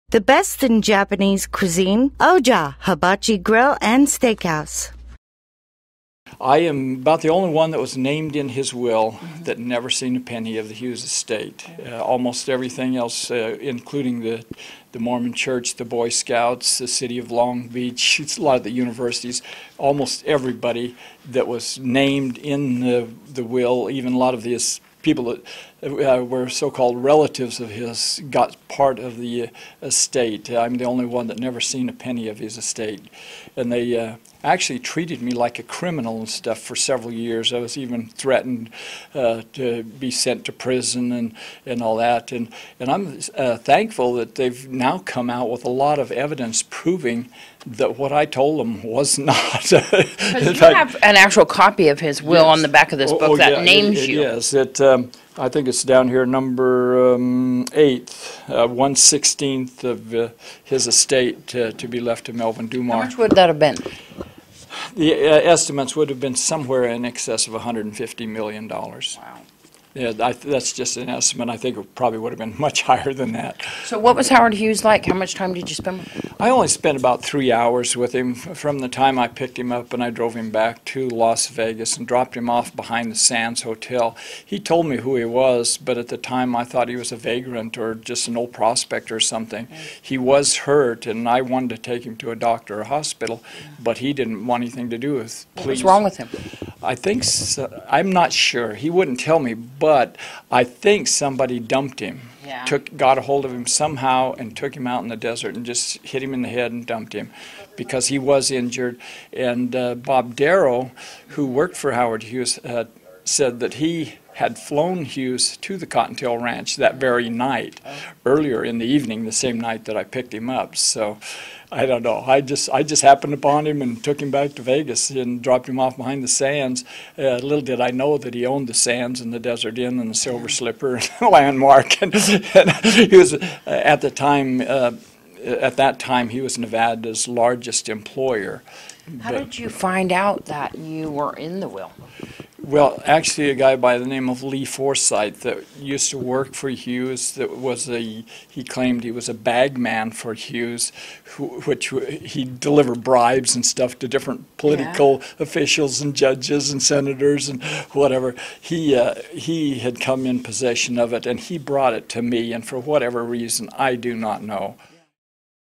07/14/2015 Melvin Dummar & Howard Hughes
Several movies have been made about him and numerous books. Similar to Ronald Wayne the third founder of Apple Computers who lives in Pahrump, fortune has slipped through his fingers, though fame has found both men here in Southern Nevada. We caught up Melvin Dummar who took Howard Hughes on a three hour drive when he needed him most and ended up in the mogul’s will.